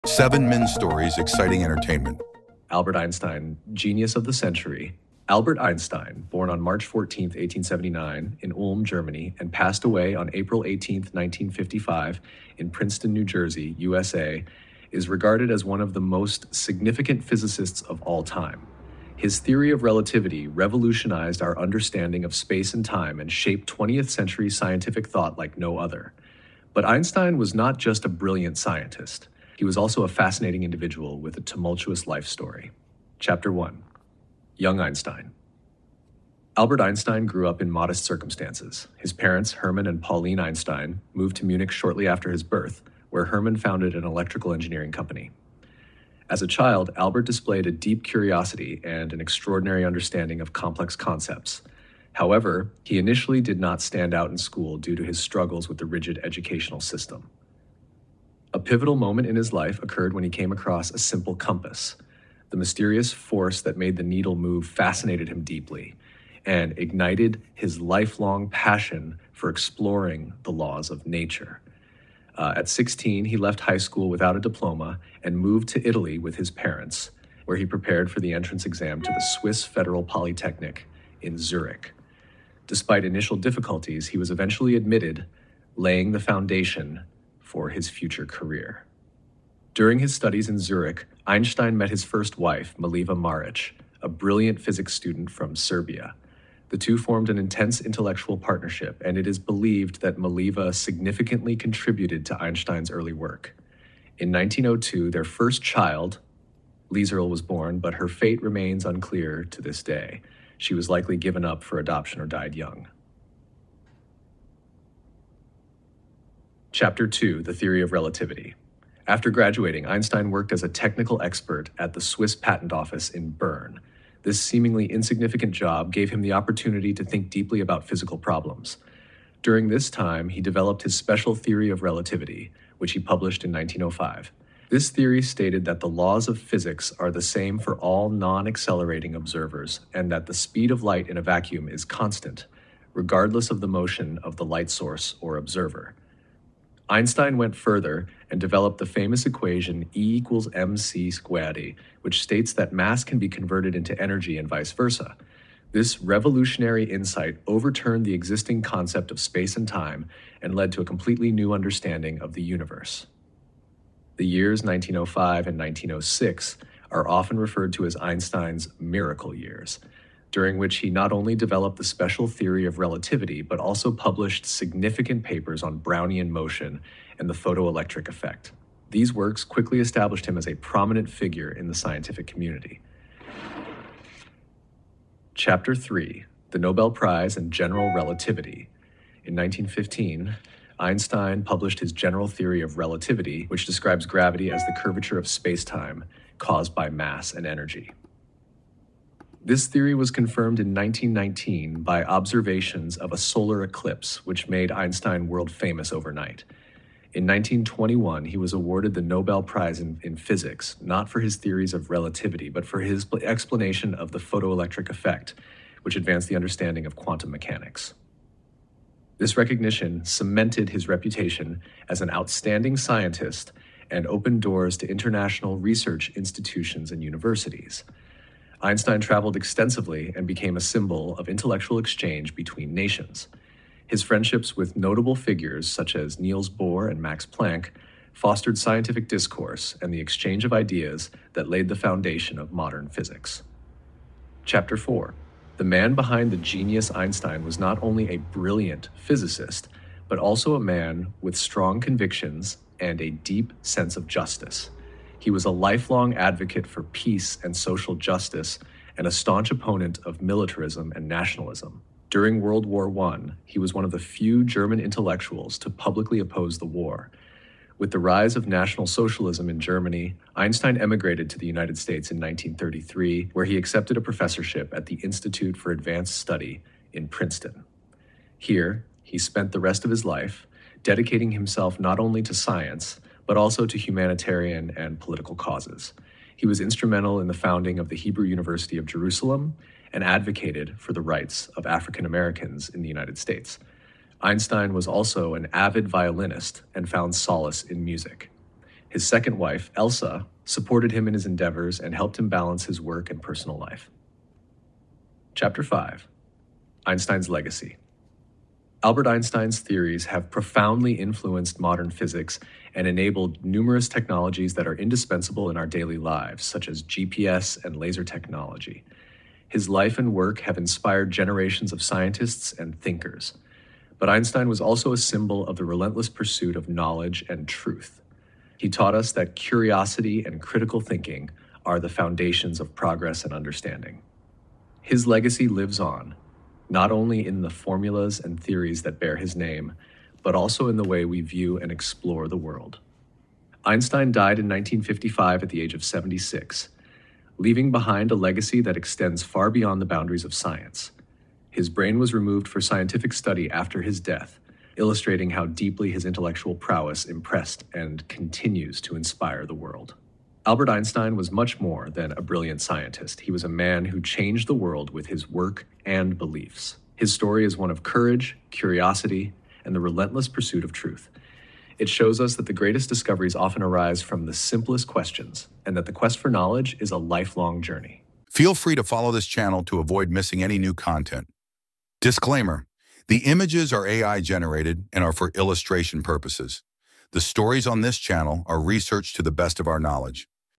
Einstein: The Mind That Rewrote Reality
Albert Einstein - Genius of the Century
Discover everything about the man who changed the world in our latest video, "Einstein's Theory of Relativity Explained in 5 Minutes!" Albert Einstein, born on March 14, 1879, in Germany and passed away on April 18, 1955, in the USA, revolutionized our understanding of space and time with his theory of relativity. His lasting impact on science is still felt today.

From his youth in Munich to his work as a patent clerk and the groundbreaking discoveries of his "miracle years," this exciting story illuminates Einstein's life and work. Learn more about his Nobel Prize award, his strong stance for peace and justice, and his role as a cultural and scientific ambassador.

Follow us to never miss any content and dive into the fascinating world of Einstein, the genius of the century. Note: The images are AI-generated and serve illustrative purposes. Our stories are researched to the best of our knowledge.

CHAPTERS:
00:00 - Introduction
00:34 - Young Einstein
01:58 - The Theory of Relativity
03:06 - The Nobel Prize and General Relativity
04:05 - The Man Behind The Genius
05:08 - Einstein's Legacy